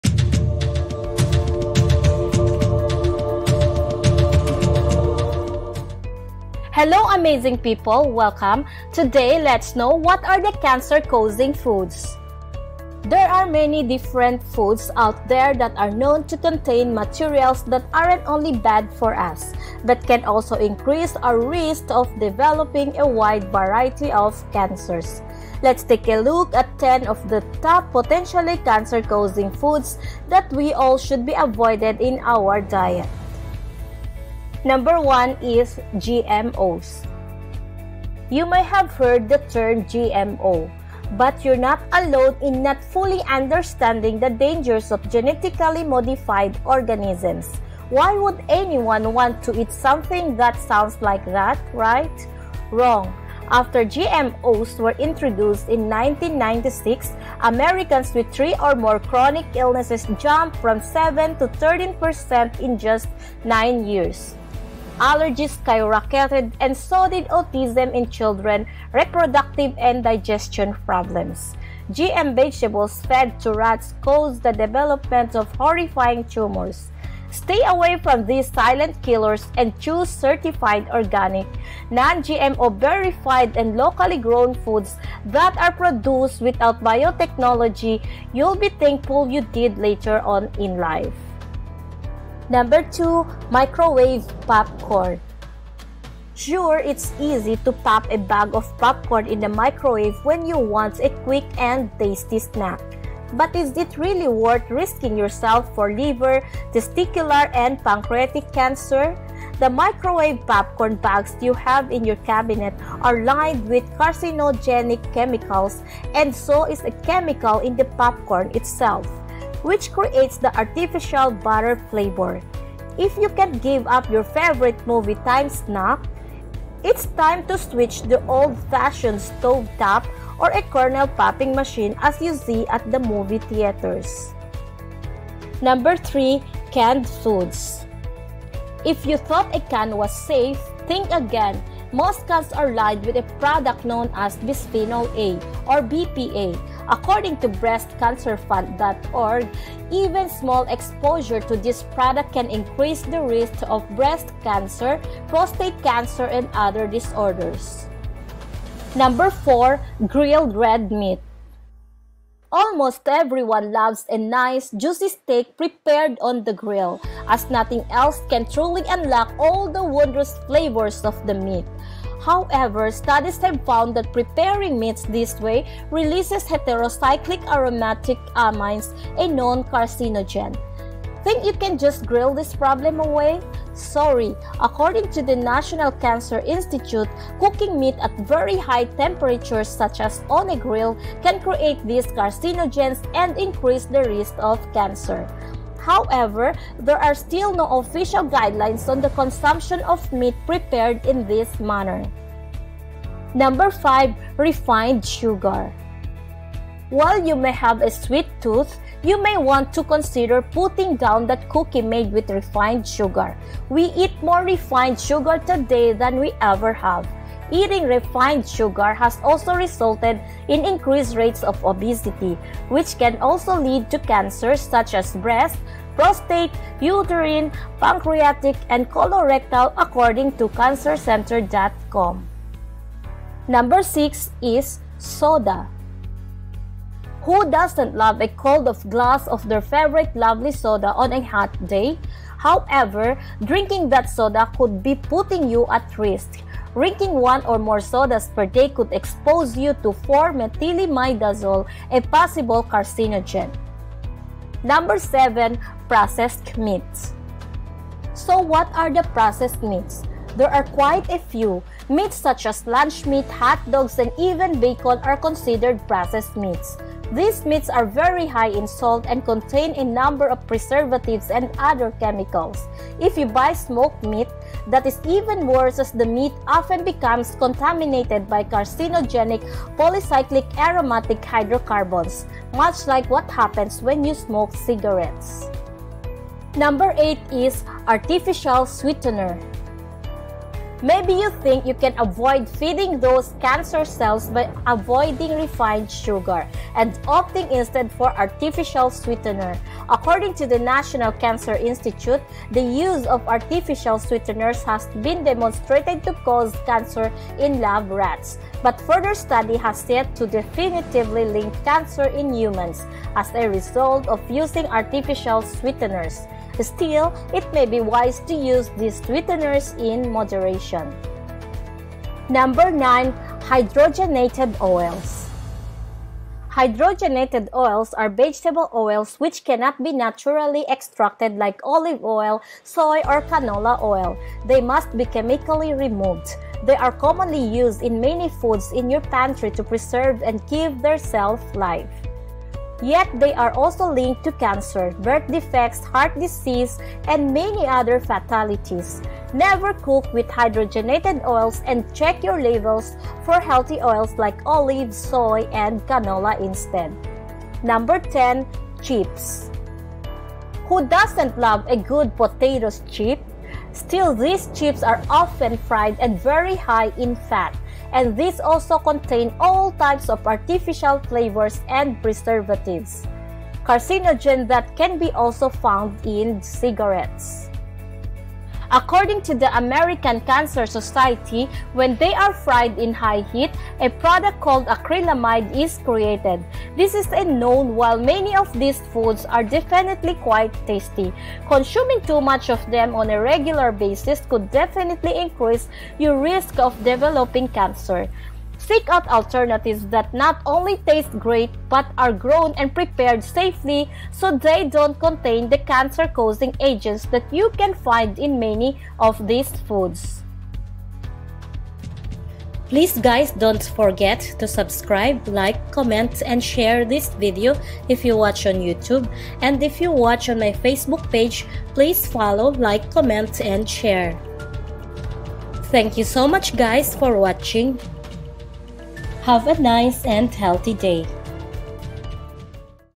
CANCER-CAUSING FOODS / WHAT ARE THE FOODS THAT CAN CAUSE CANCER?
CANCER-CAUSING FOODS
WHAT FOODS CAN CAUSE CANCER?
There are many different foods out there that are known to contain materials that aren’t only bad for us but can also increase our risks of developing a wide variety of cancers. Let’s take a look at 10 of the top potentially cancer-causing foods that we all should be avoided in our diet.
To improve your channel pls click this To create stunning video fast pls click this